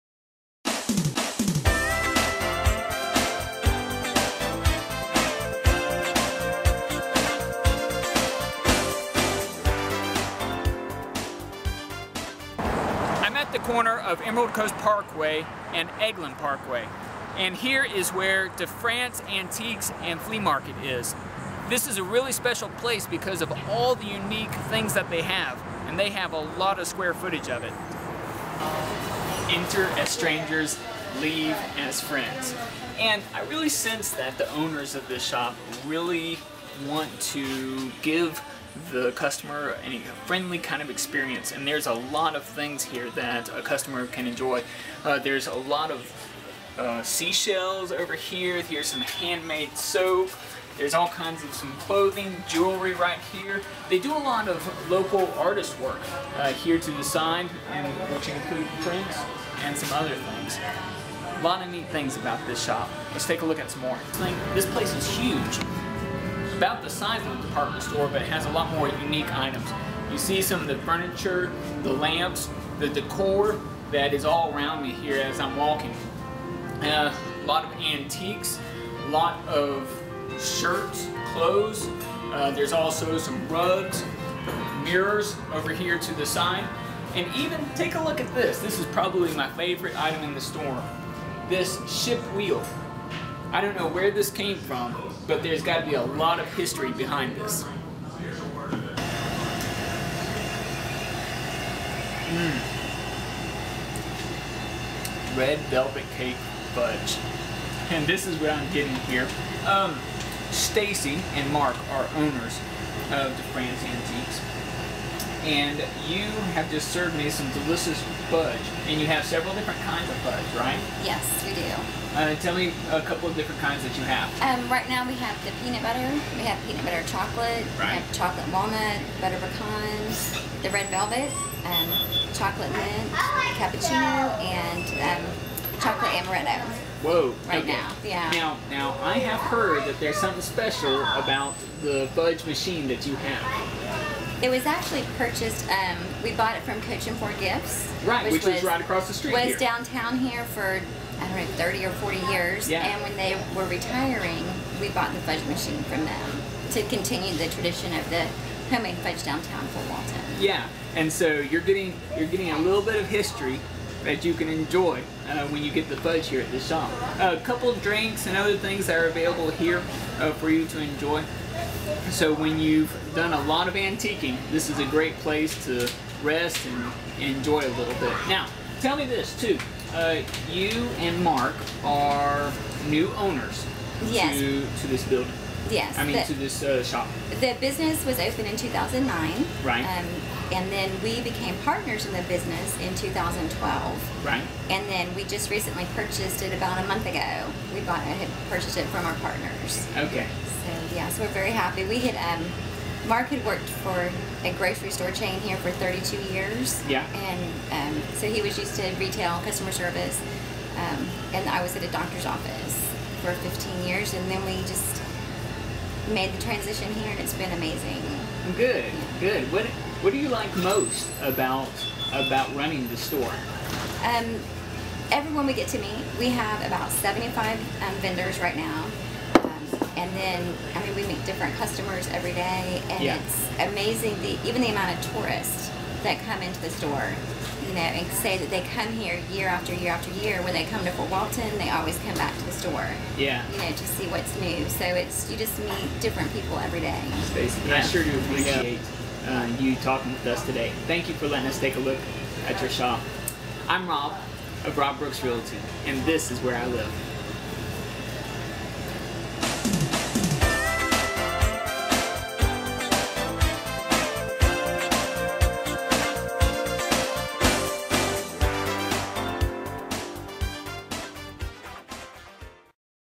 De' France Indoor Flea Market and Antiques: 10 Coolest Shops of Fort Walton Beach
If you love flea markets and antiques then you will be heaven when you find out that De' France has a space of 13,000 sq feet of anything you could imagine! What will make you more excited is learning that inside this amazing antique store, located right off of Miracle Strip Parkway in Fort Walton Beach, is a homemade fudge shop.